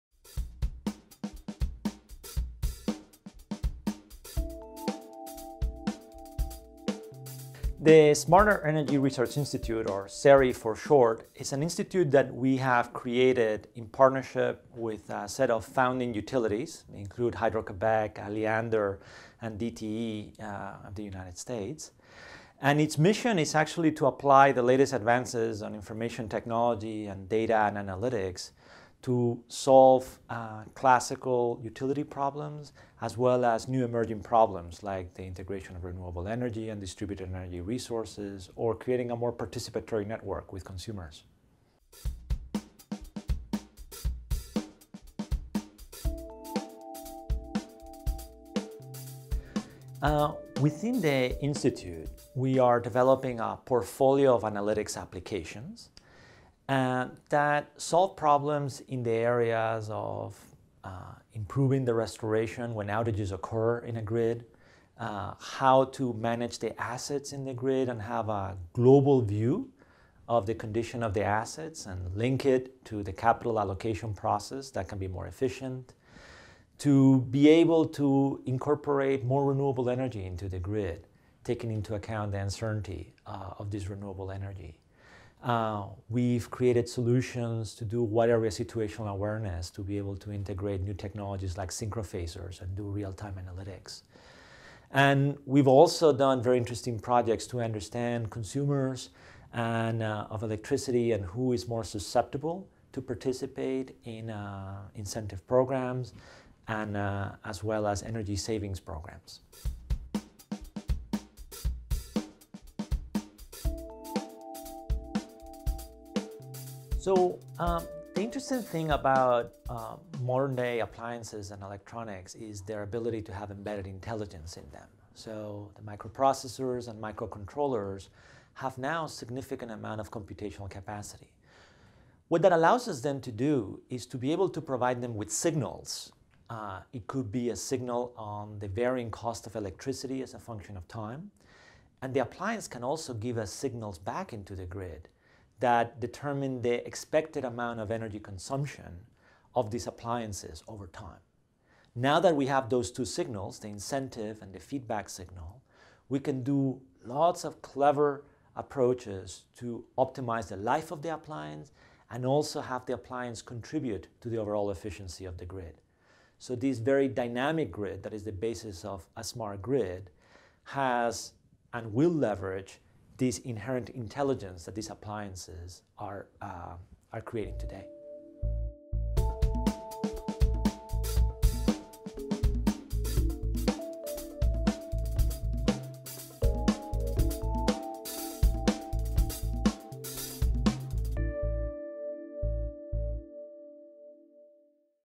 Creating the Utility of the Future - Dr. Dario Gil - 16 October 2013
About the Speech:

As energy markets move to integrate more and more intermittent resources, Dr Gil is leading an international research consortia focused on improving the technology used in energy markets, in such areas as: providing a predictive weather service, creating a more responsive transmission grid, and stablising the use of renewable energy sources. In this address he will provide listeners with an insight into the work undertaken by the SERI in its mission to create the utility of the future. This is the ninth lecture in the ESB Energy Lecture Series.

About the Speaker:

Dr Gil is the Director for Energy & Natural Resources at IBM Research, the Director of the SERI, New York, and a leading technologist and executive at IBM. As the Creator and Founding Director of the SERI, he is responsible for leading an international collaborative research consortia focused on advancing the utility of the future. As a Director at IBM Research, he has worldwide responsibilities for IBM's energy and natural resources research activities, leading a global team of scientists across 12 laboratories, focused on accelerating industry transformation through breakthrough science and the systematic application of the latest advances in information technology.